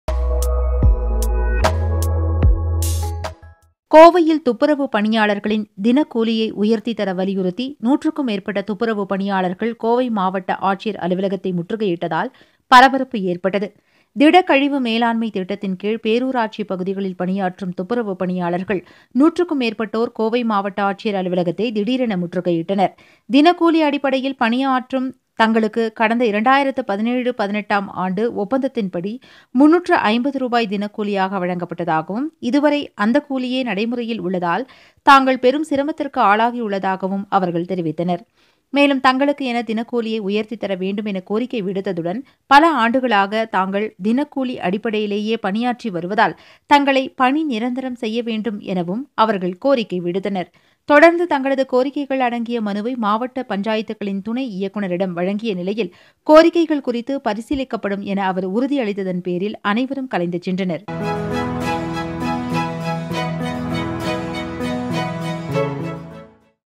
கோவையில் மாவட்ட ஆட்சியர் அலுவலகத்தை முற்றுகையிட்ட துப்புரவு பணியாளர்கள்
கோவையில் துப்புரவு பணியாளர்களின் தினக்கூலியை உயர்த்தி தர வலியுறுத்தி நூற்றுக்கும் மேற்பட்ட துப்புரவு பணியாளர்கள் கோவை மாவட்ட ஆட்சியர் அலுவலகத்தை முற்றுகையிட்டதால் பரபரப்பு ஏற்பட்டது.